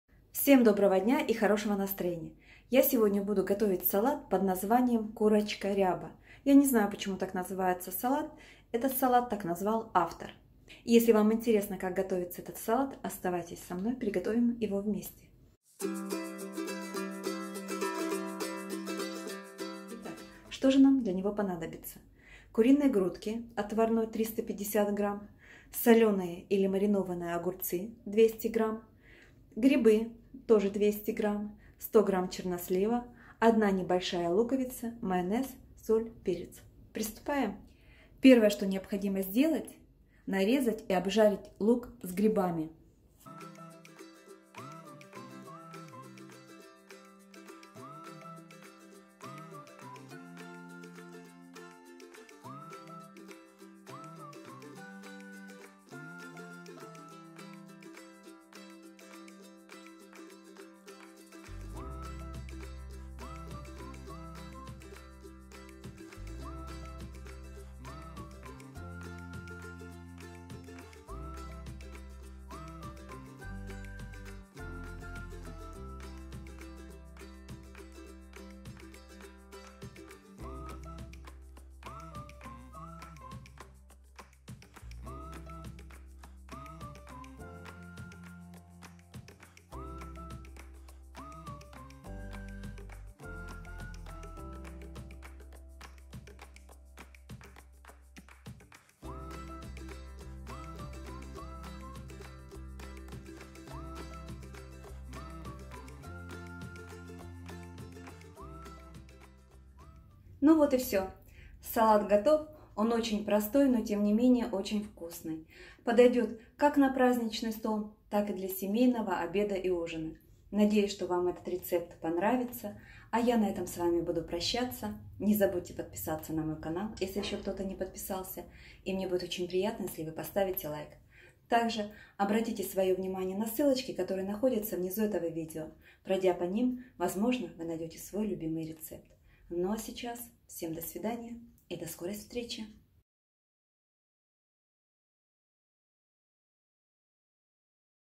Вот новый Салат// Выпросила рецепт в кафе// Все кто у меня пробовал просили Рецепт// САЛАТЫ РЕЦЕПТ//
Вот новый Салат. Выпросила рецепт в кафе. Все кто у меня пробовал просили Рецепт.

Ингредиенты:
Куриная грудка -350гр
Огурцы маринован-200гр
Грибы-200гр
Чернослив-100гр
Лук-1 небольшая
Майонез, соль, перец